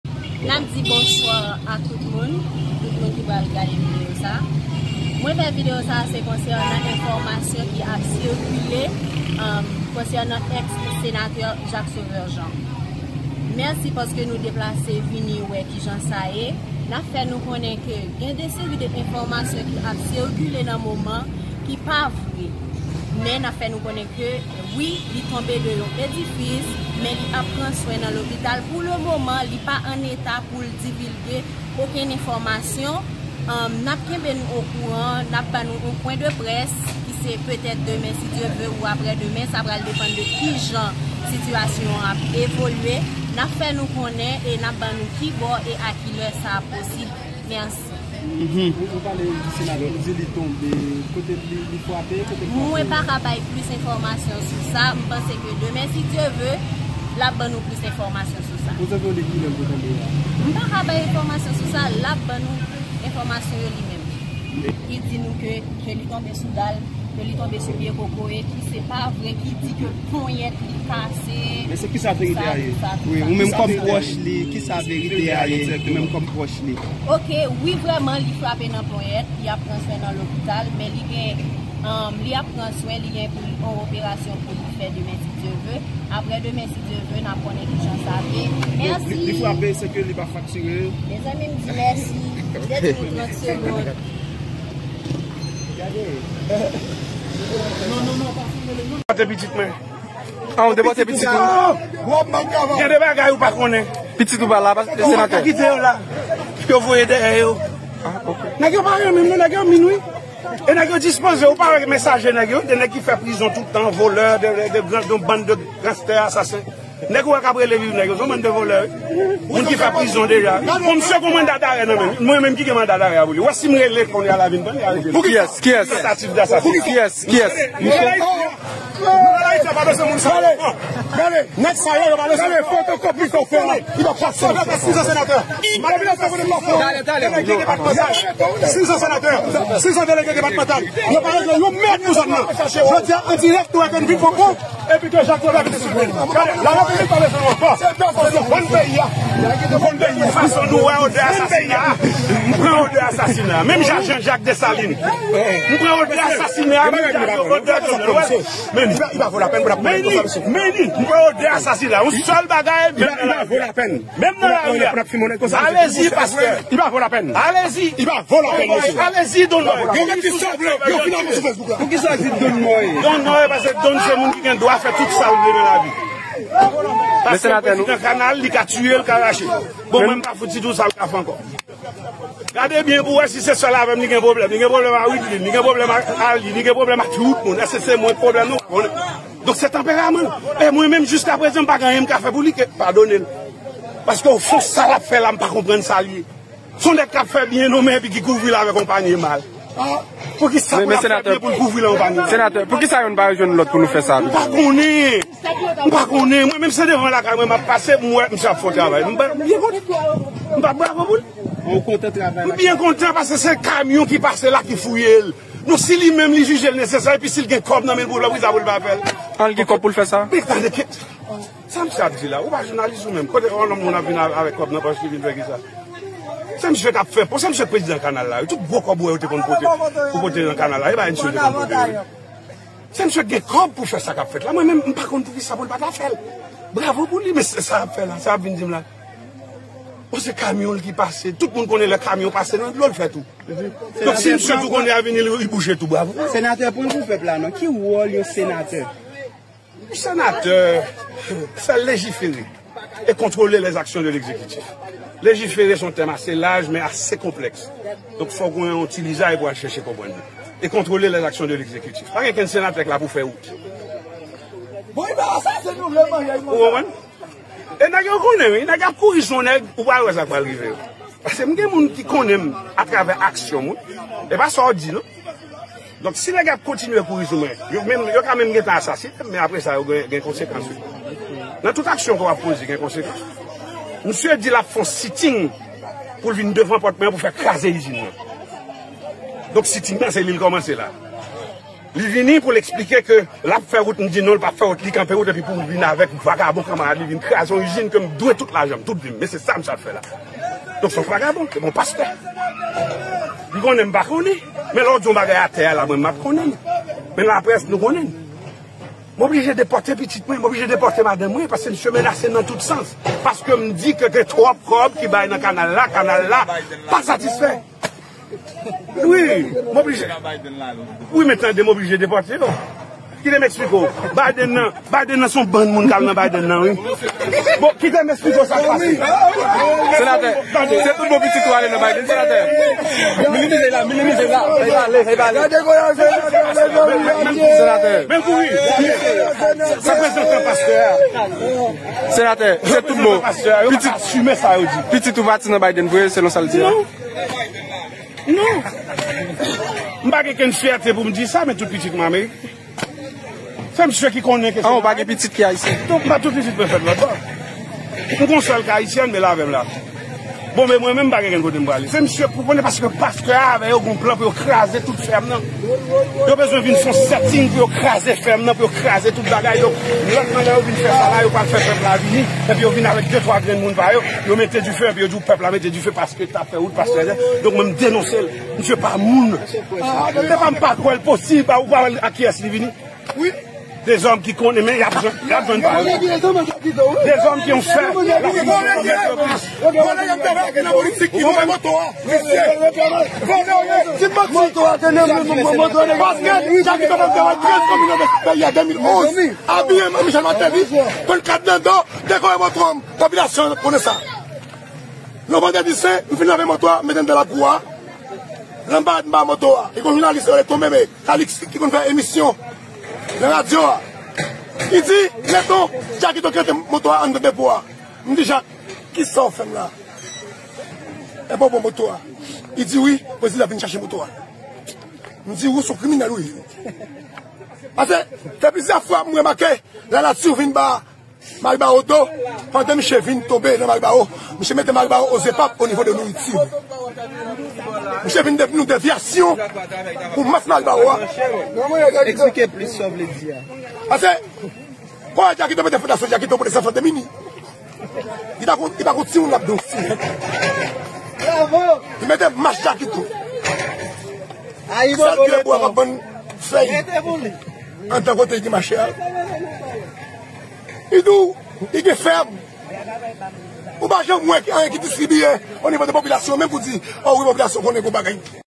Breaking News!Jakito sot Tonbe anlè do yon kay, li grav,ponyèt kase li lopital.Priye pou Senatè a
Please kontibye Abòne @RadioTambouATL la tranding